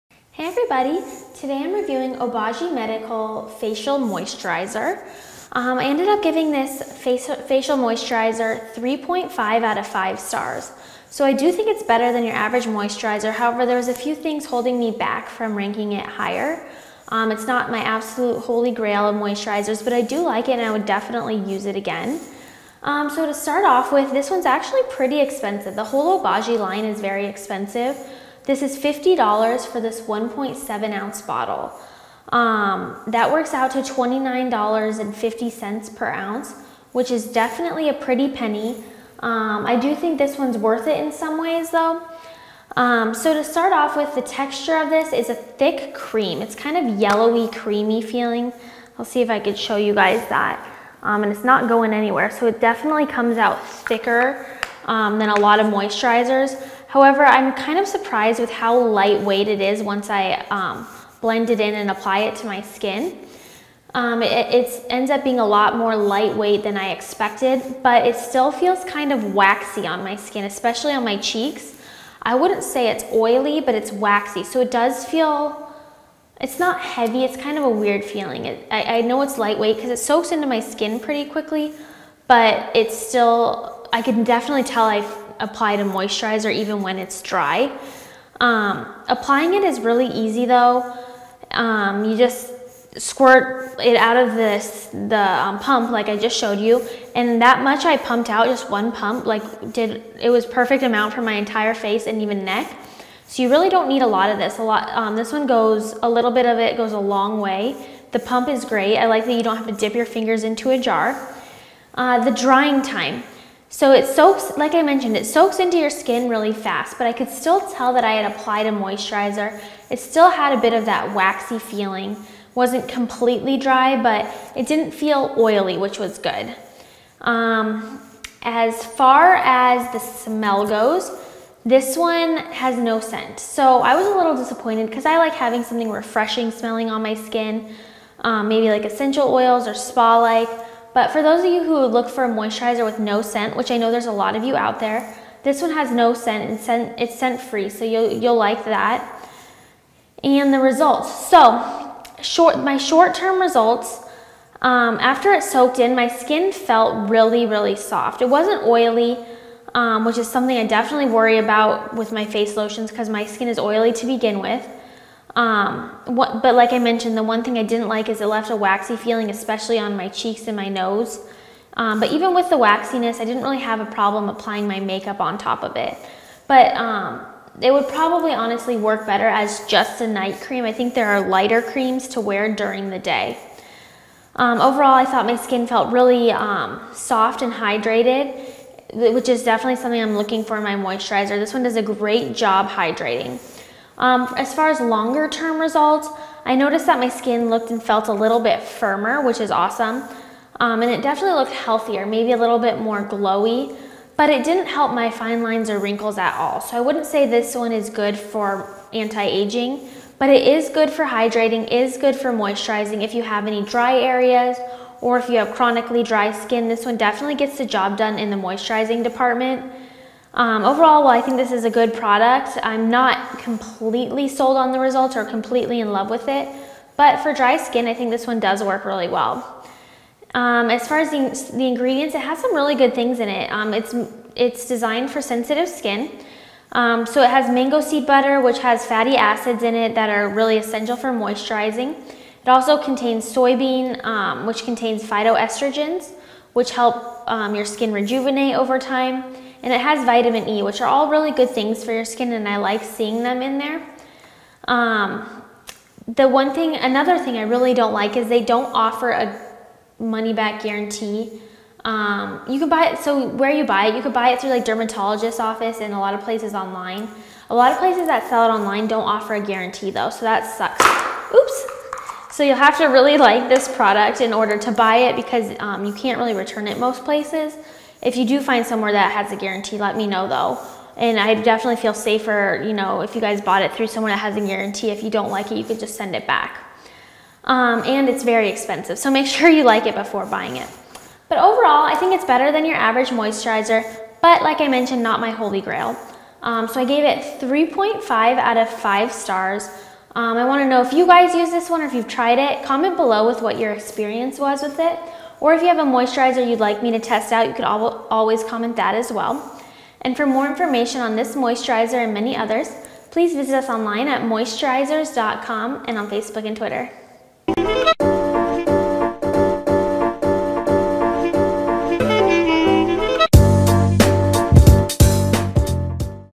Obagi Medical Hydrate Facial Moisturizer Review
After this review, be sure to check out our list of the top 10 Moisturizers!

Want to know what I think of this moisturizer?  This is a comprehensive review of Obagi Medical Hydrate Facial Moisturizer. See what real experts and actual users have to say about moisturizing product!

People are always asking me which moisturizer is best.  In this review I’ll go over everything you need to know about this product.

Here's a breakdown of what you'll learn:

First, I’ll give you my overall rating of the product based on how it stacks up to all the other moisturizers I’ve tried.  You don’t want to miss this part!

Then, I talk about the texture and feel of it.  Moisturizers can come in many forms such as gels, creams, lotions, etc.  Each one is different, and I’ll tell you my first impressions on how it feels and looks.

Next, I talk about the smell, which is very important as you’ll be applying this product to your face. Some of these moisturizers smell bad, and I’m here to try them out so you don’t have to.

Most importantly, I go over the results I saw by using this moisturizer.  If you’ve ever wondered, “ Ok, there are thousands of moisturizers, but which ones really work?” then you’ll want to stick around for this part.   And lets just say I am brutally honest about which products I do and don’t like.

At the end of my Obagi Medical Hydrate Facial Moisturizer review I’ll tell you if I think this moisturizer is worth it, all things considered. You don’t want to miss my overall opinion so make sure to stick around!

In conclusion, if you want to learn all about this moisturizer, I’d recommend checking out this video.

And here's our list of the 10 best face moisturizers again!